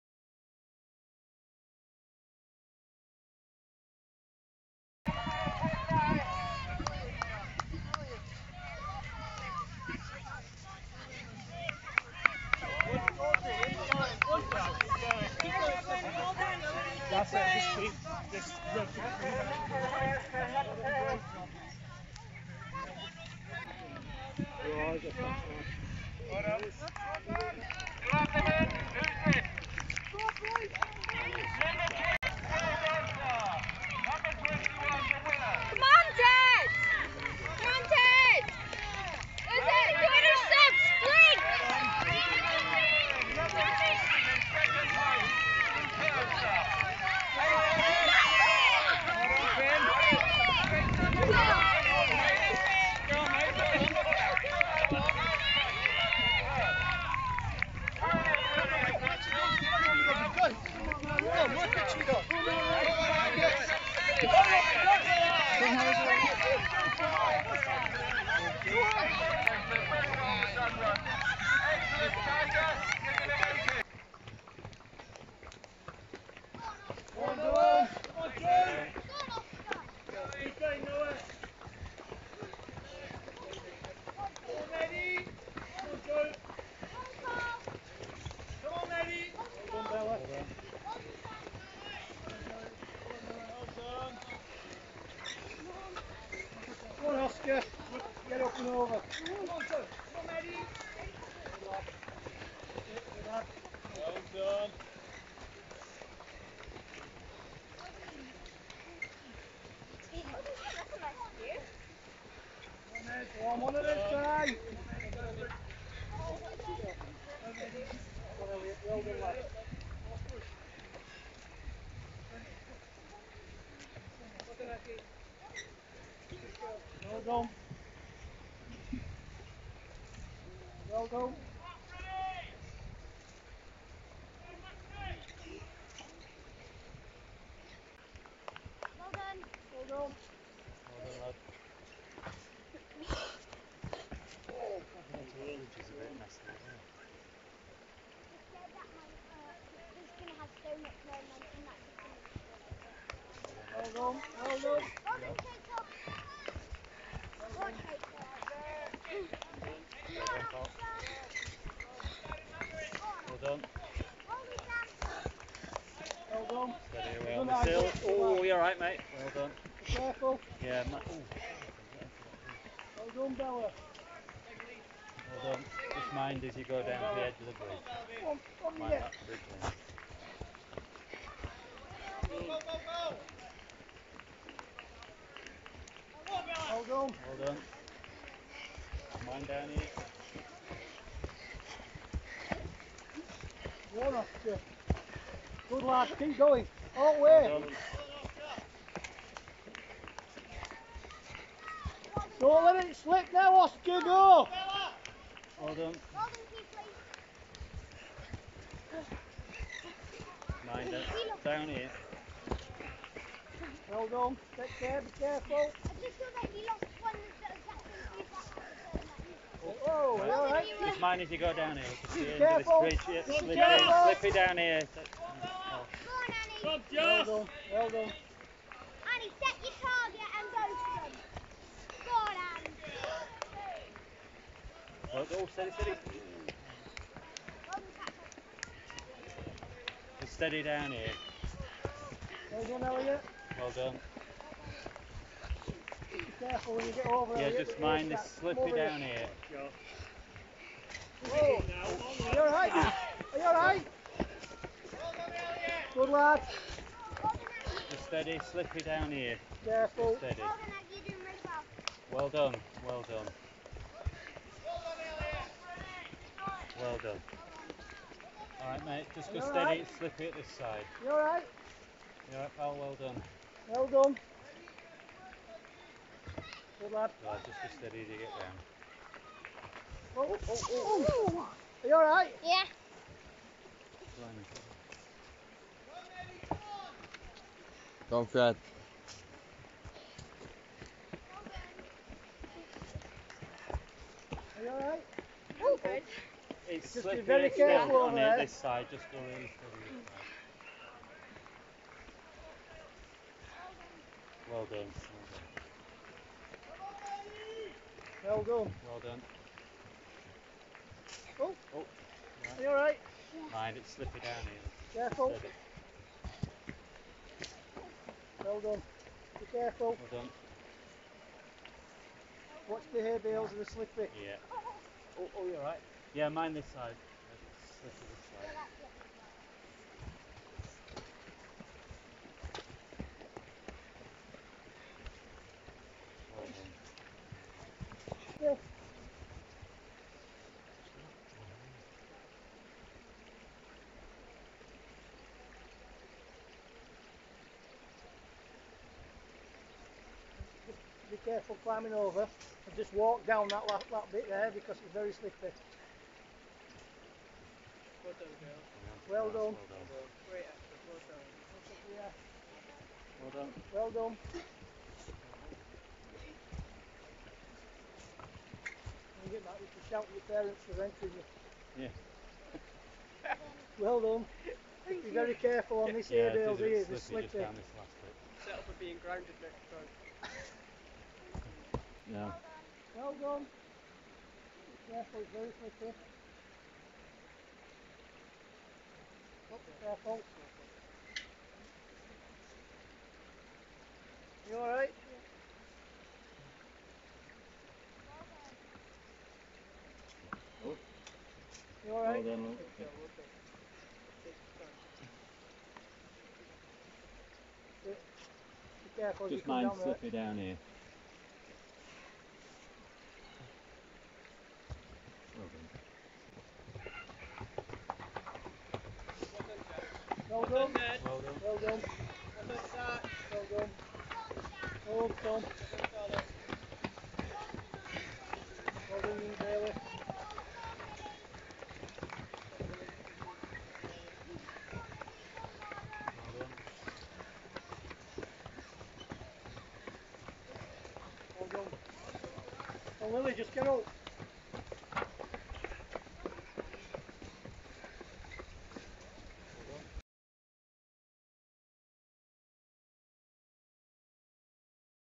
BOFRA Hebden Sports 2021 - Junior Races
The under 9 to under 14 races at Hebden Sports.